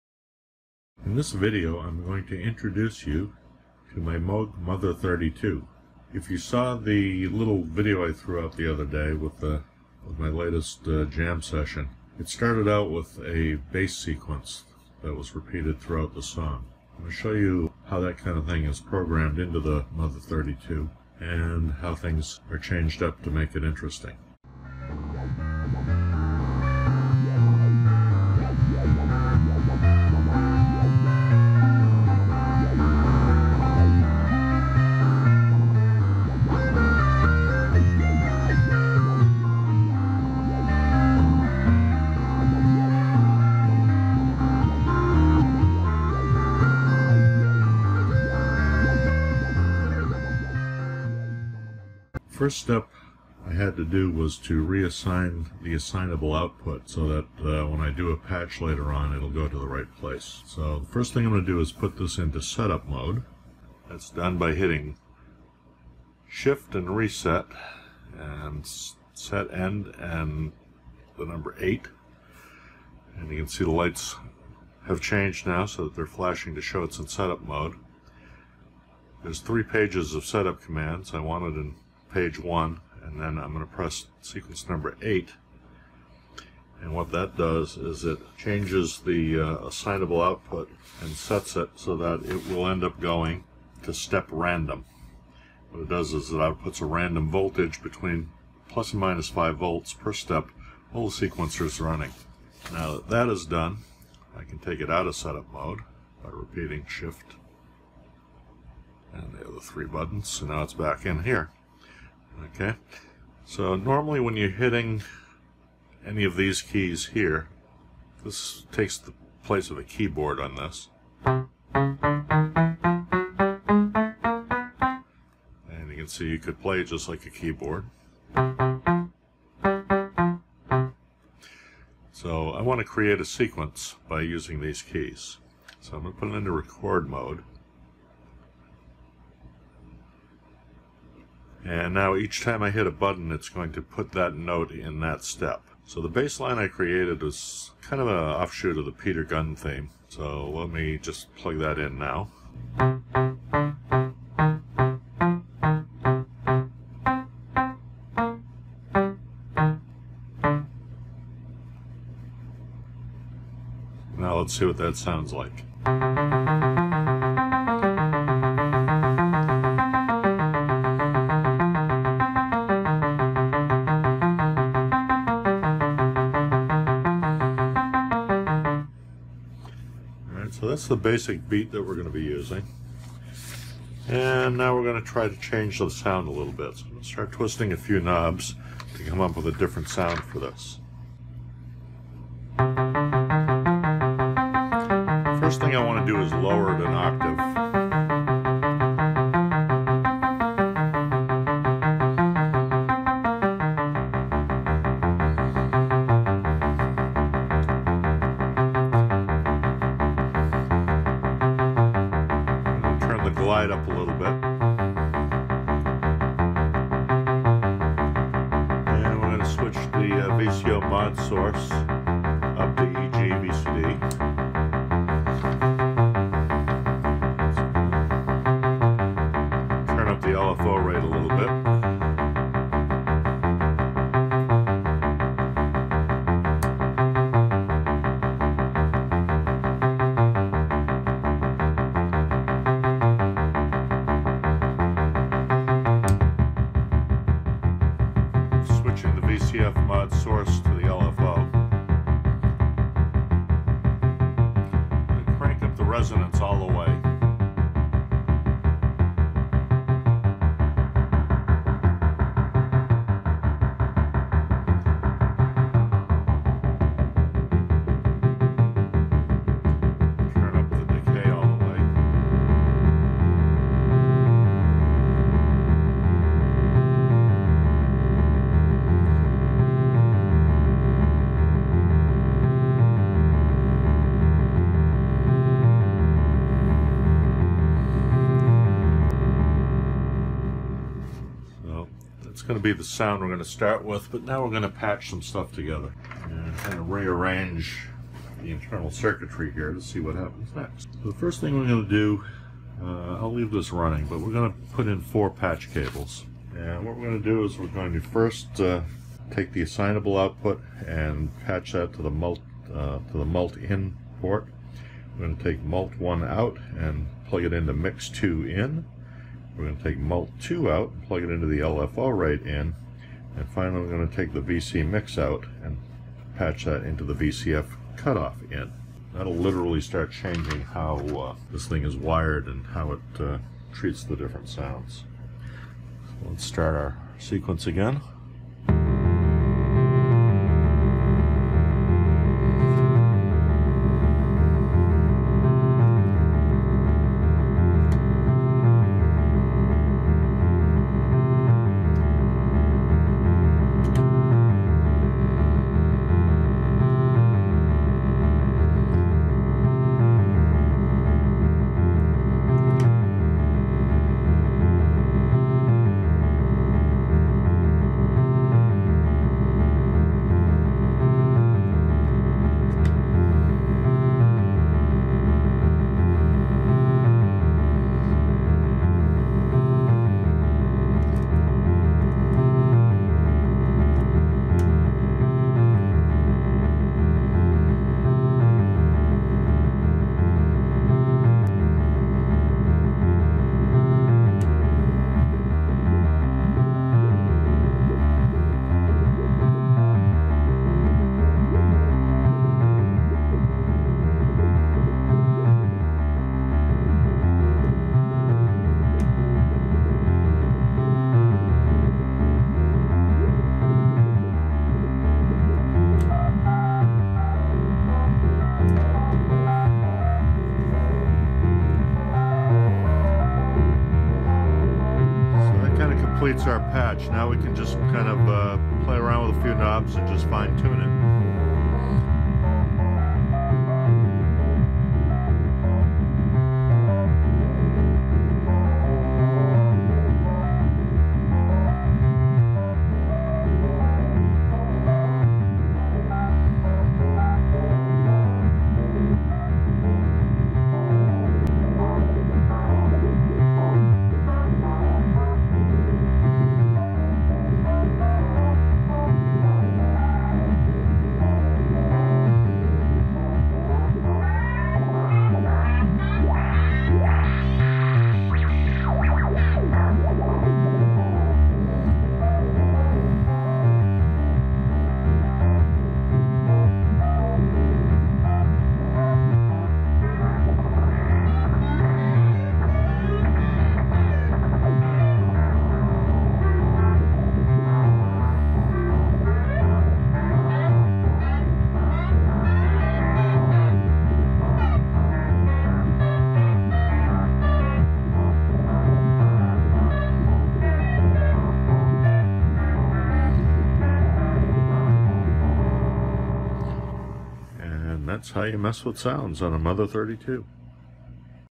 Moog Mother 32 Sequencing Demo
Live example of how to set up a sequence on a Moog Mother 32.